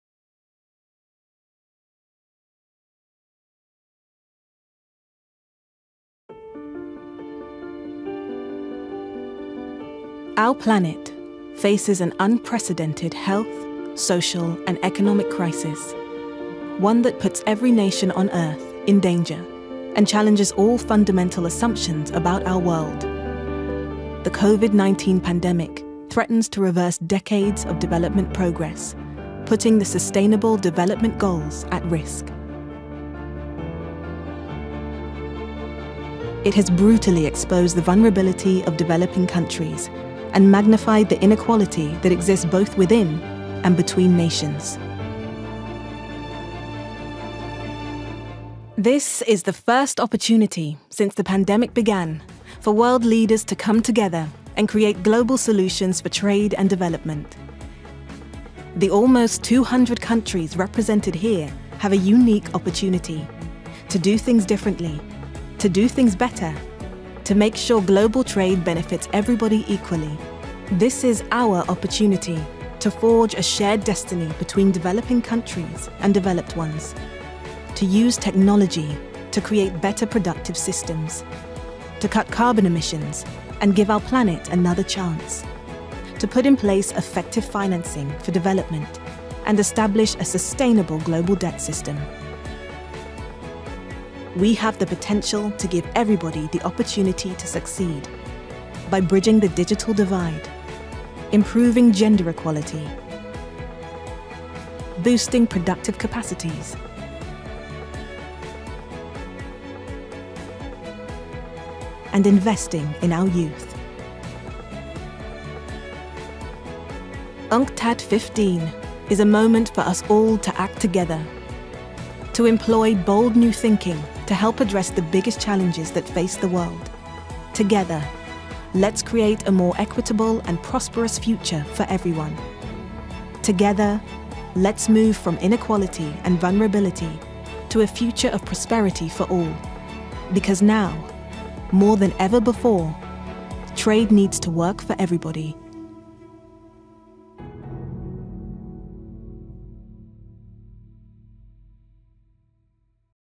UNCTAD15: A chance to build back differently, better, and ensure trade works for all
Today UNCTAD brings together nearly 150 countries amid a crisis unlike any we have seen in our lifetime.

UNCTAD15 provides the first opportunity since the start of the pandemic for world leaders to come together and generate global solutions for trade and development.

It is a chance to build back differently, to build better, and to ensure trade works for everybody. Join us.